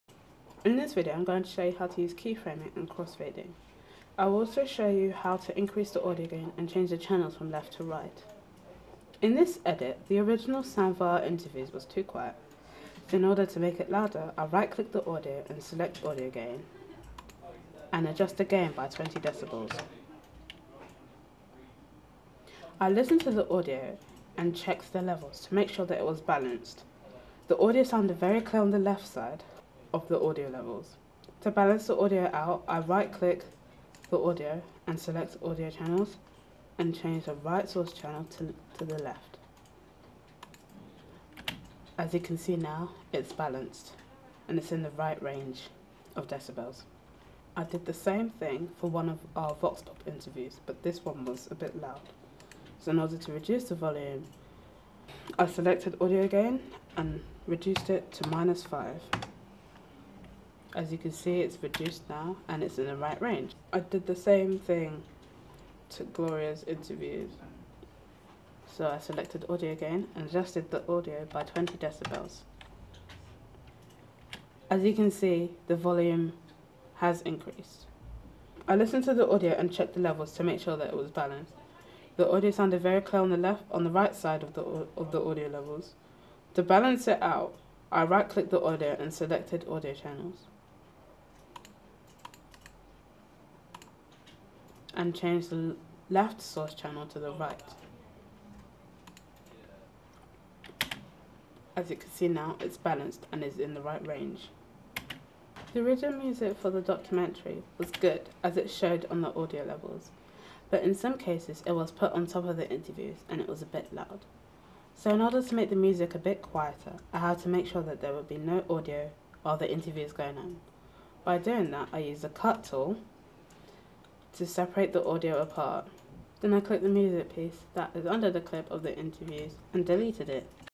Mental Health Documentary Sound Edit
This is my sound edit for my mental health documentary. In this video I will show you how to use the key frame and the audio transitions